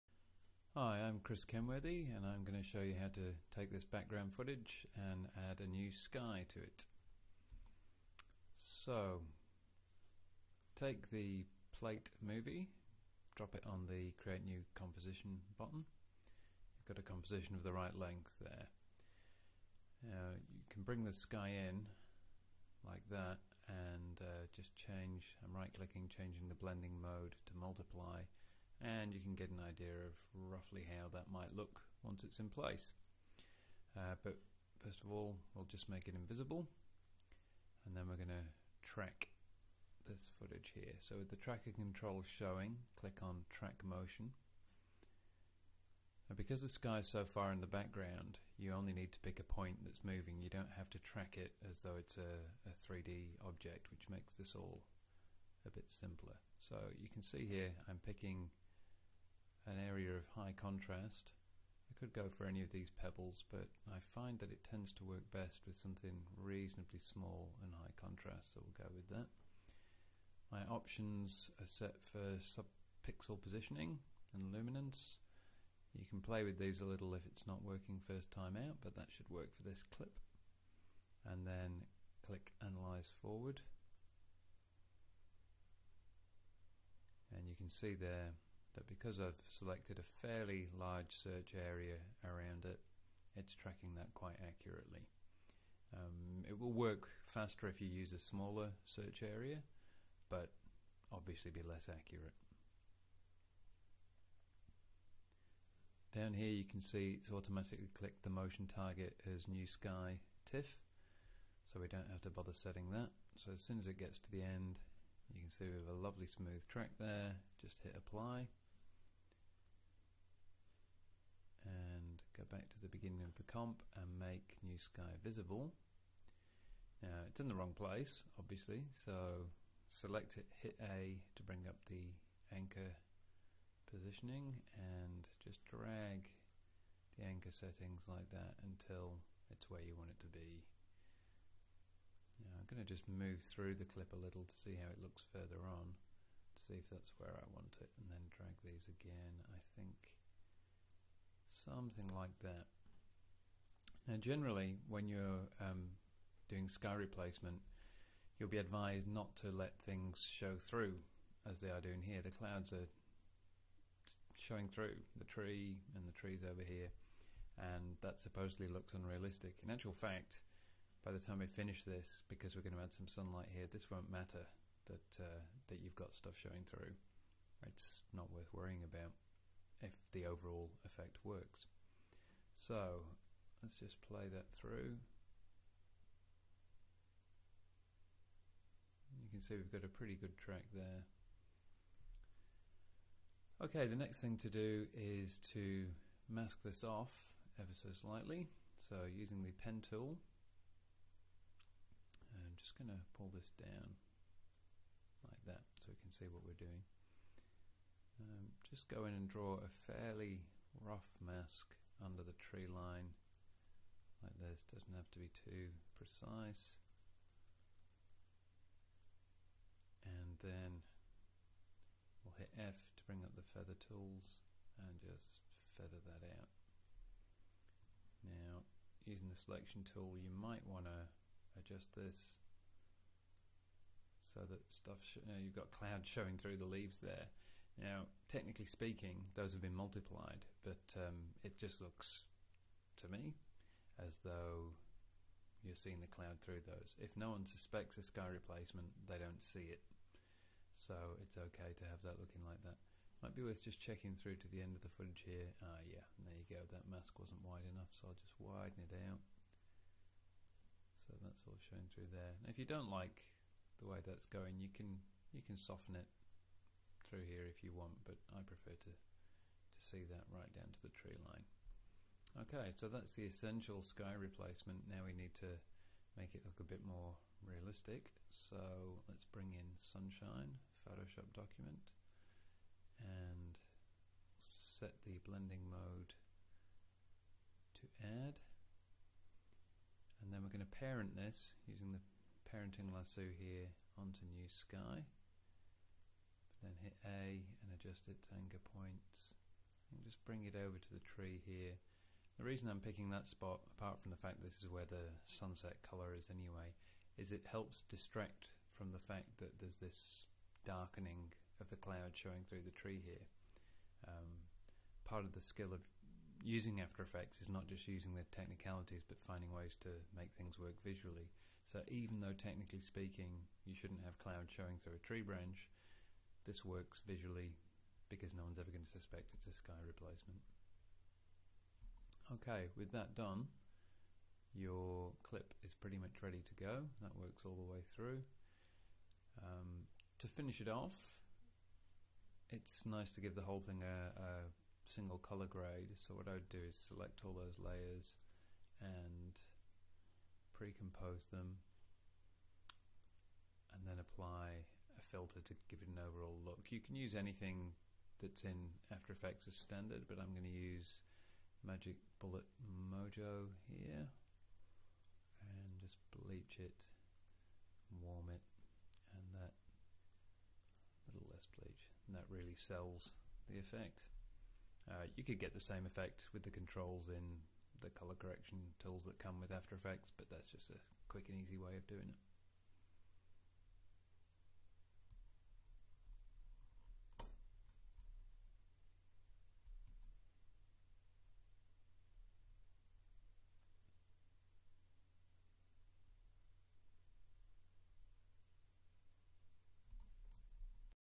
Video tutorial: Replace a dull sky in After Effects
Christopher Kenworthy shows you how to create that post-apocalyptic look with a rich, dark atmosphere